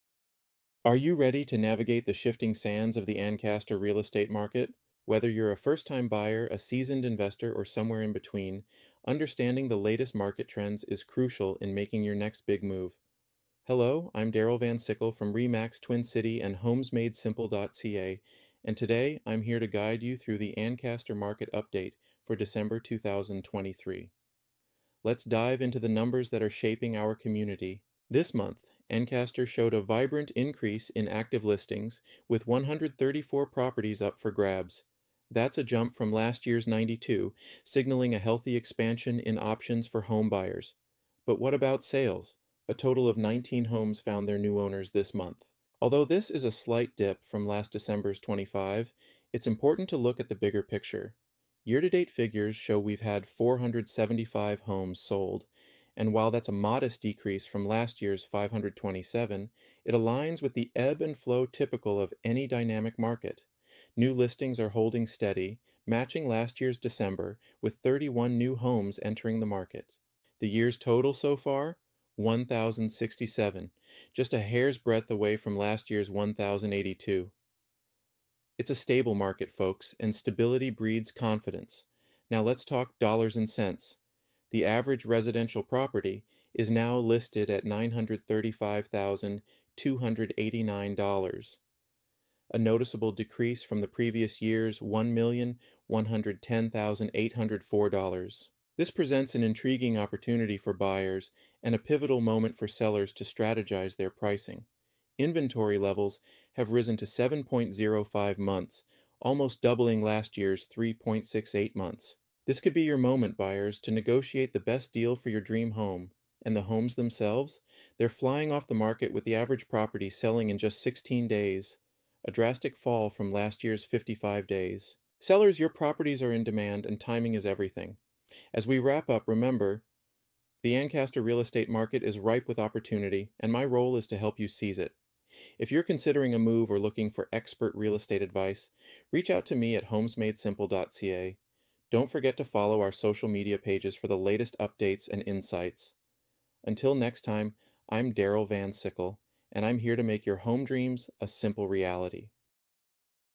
Ancaster Real Estate Market Update - December 2023
In this video, we'll explore the Ancaster Real Estate Market Update and December 2023 Trends. Join Darryl Van Sickle from ReMax Twin City and Homesmadesimple.ca as he provides an in-depth analysis of the current state of the market, including active listings, sales data, and average property prices. Discover what these trends mean for buyers, sellers, and investors as we head into the new year.